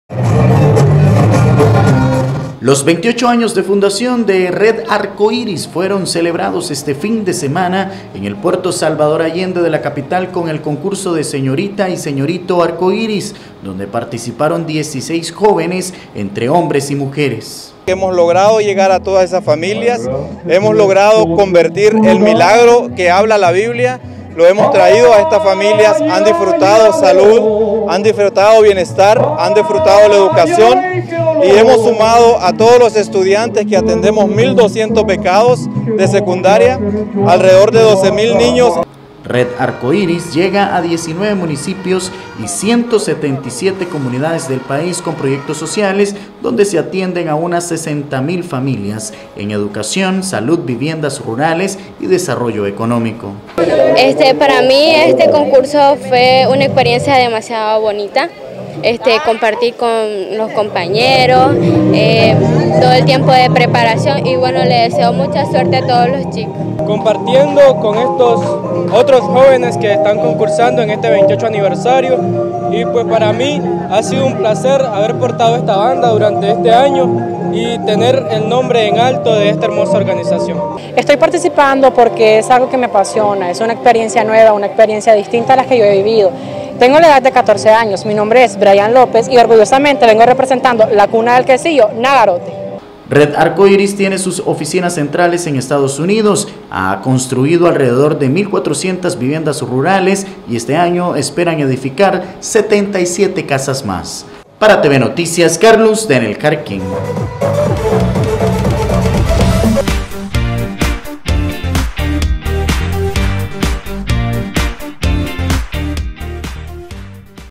Red Arcoíris celebra 28 años de fundación en Nicaragua
La Organización Sin Fines de Lucro, Red Arcoíris, celebró su 28 aniversario de fundación en el Puerto Salvador Allende, donde se eligió a una señorita y un caballero que representará a dicho organismo este año.